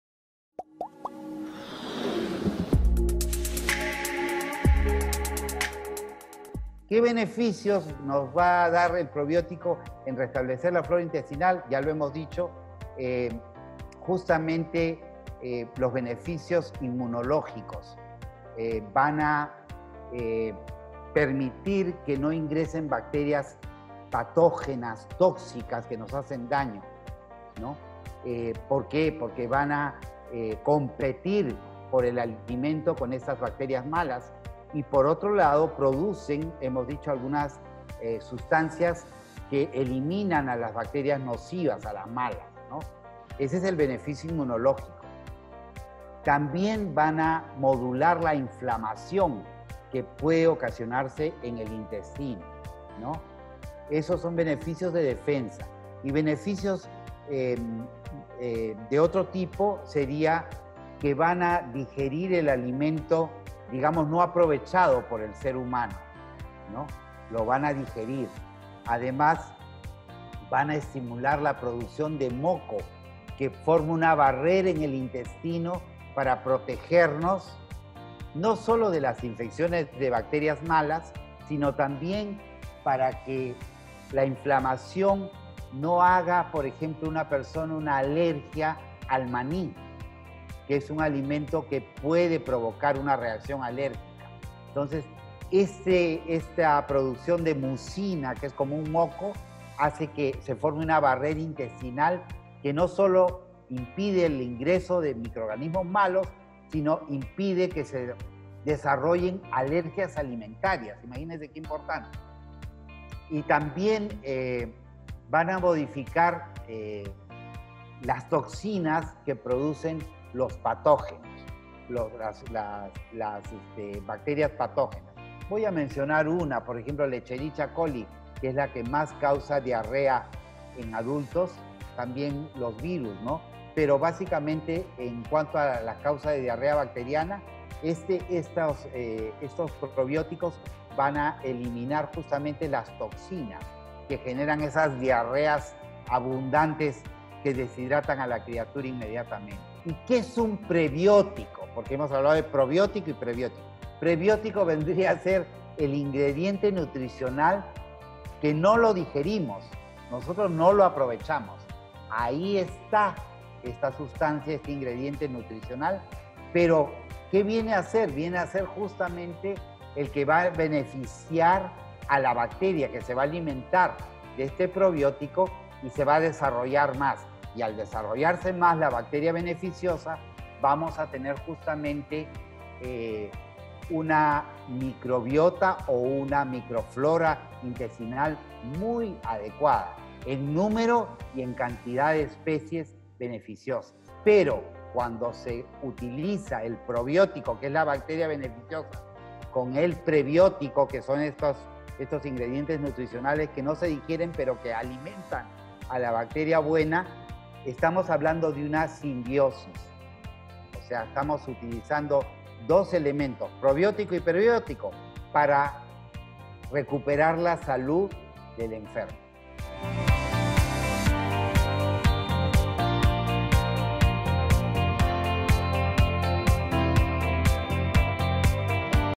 MITO O VERDAD: "¿SON BENEFICIOSOS LOS PROBIÓTICOS Y PREBIÓTICOS PARA EL CUERPO?"
Al respecto, el Dr. José Cano, Pediatra, nos responde la pregunta.